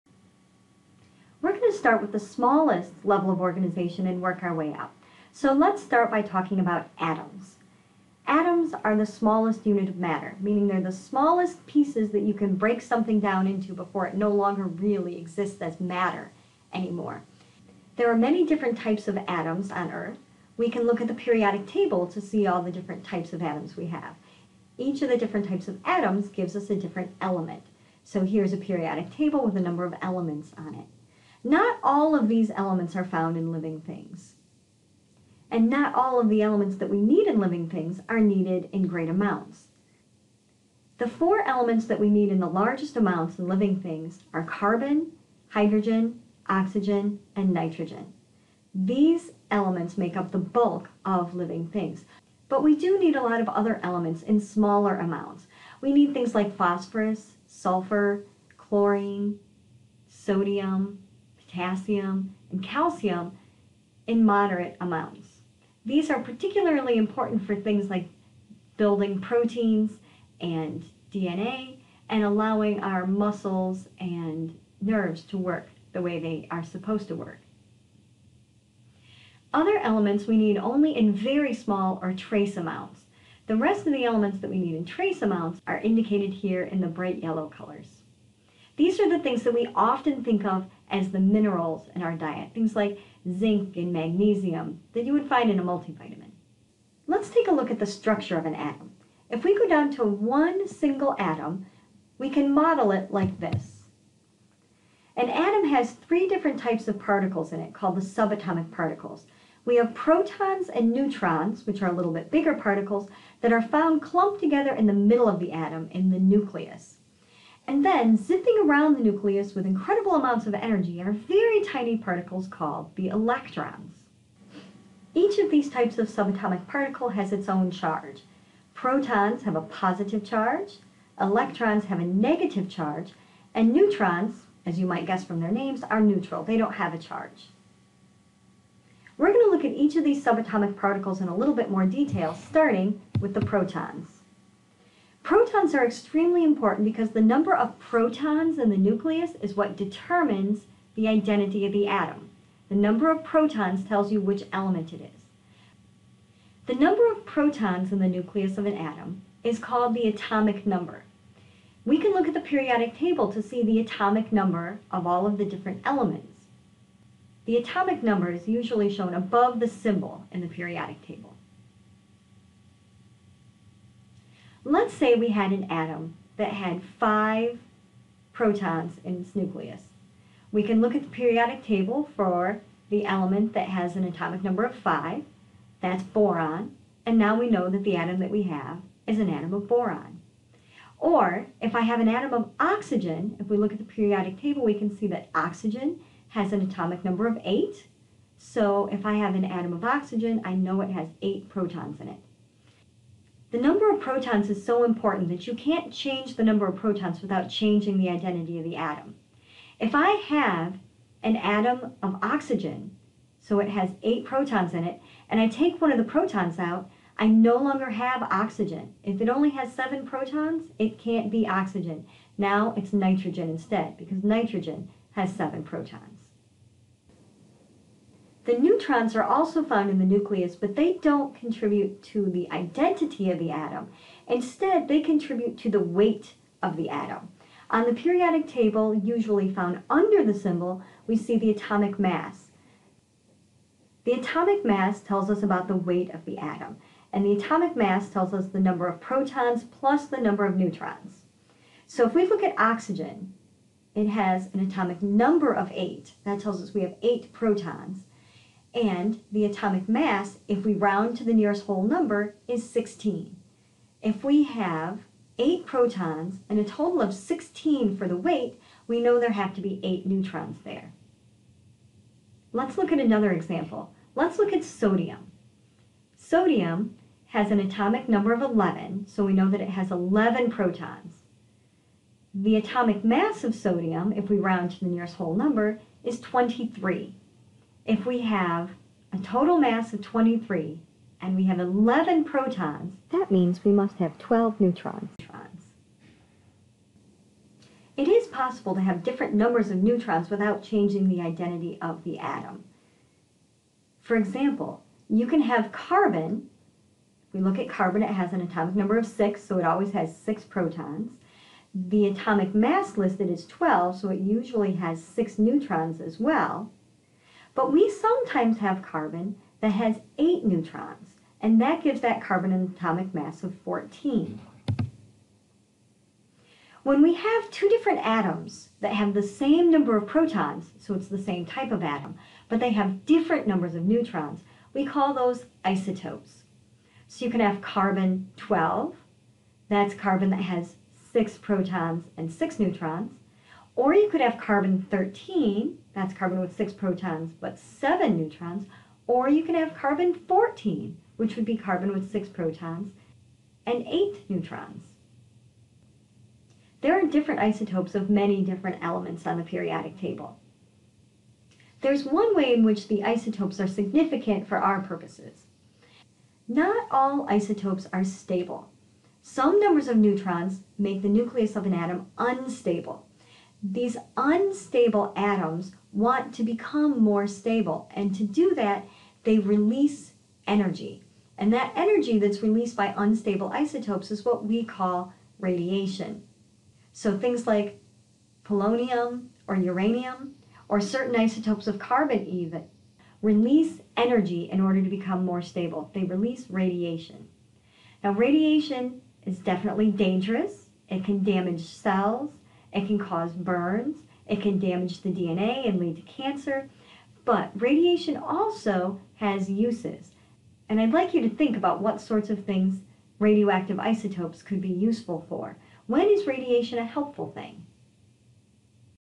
AtomsPart1
BIO 271 with Dr. Fritz at St. Clair County Community College in Port Huron, MI

Atomic structure, protons, and neutrons